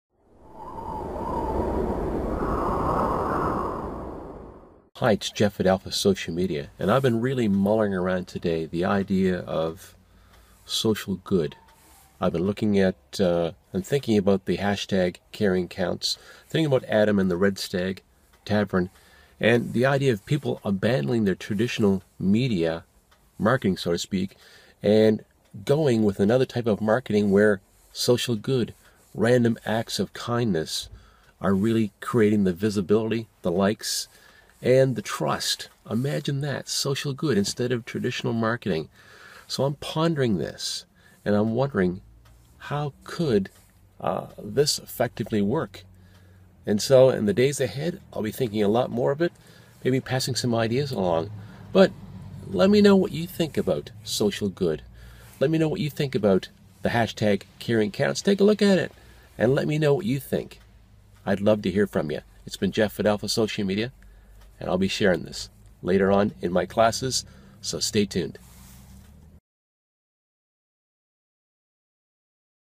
Social Good Marketing, Can It Replace Traditional Media Marketing?
Did you ever consider dropping your traditional media marketing for social good marketing? What would happen if you dumped your traditional media marketing and opted for this type of marketing? Adam at the Red Stag Tavern in Halifax is doing just this.